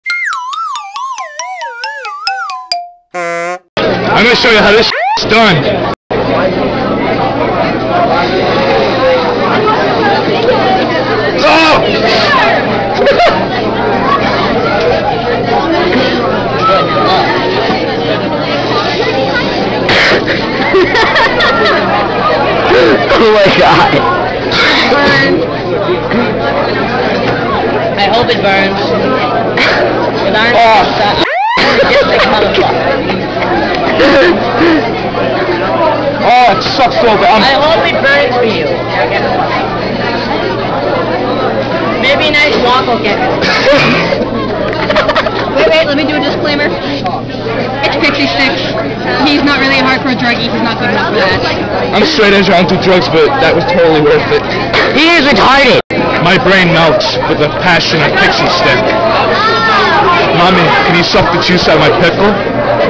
How not to eat a pixie stick
I feel so bad but it's ok because Huli said it was worth it. So it's ok now. How not to eat a pixie stick.